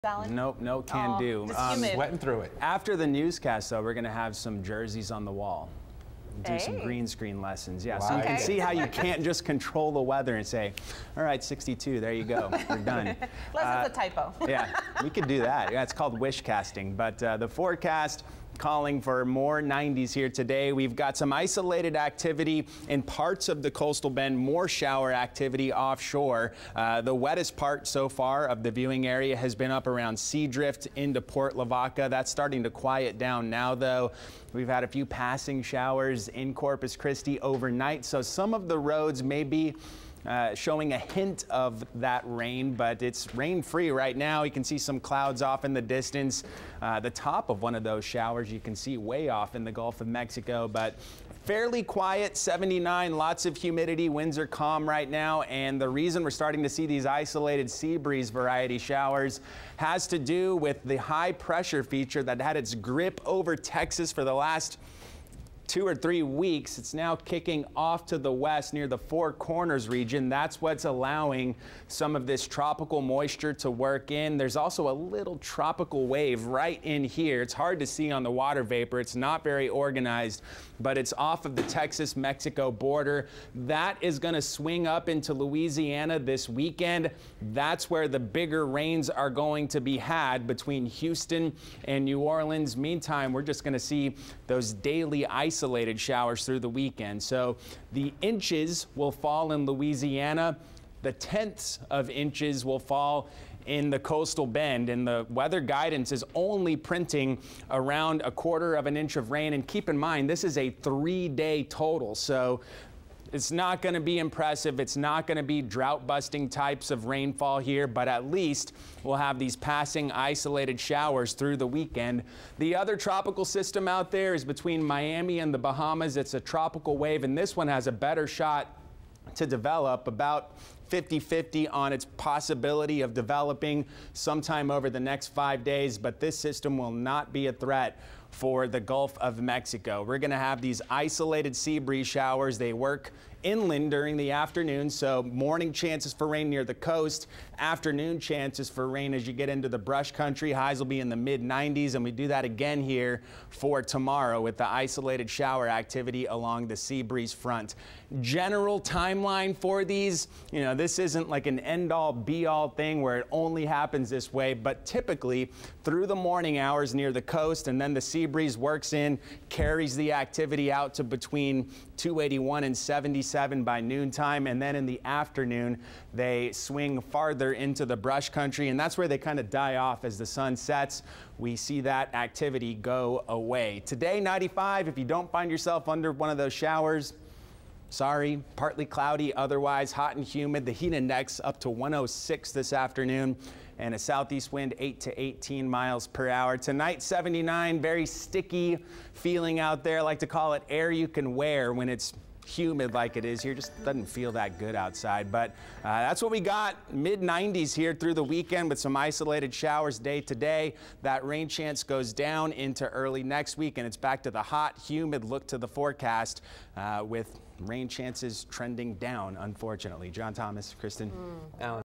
Alan Holt KIII South Texas Weather Forecast 08-23-2019
Isolated showers will be possible each day through Sunday.  Then rain chances trend down with temperatures topping out in the middle 90s next week.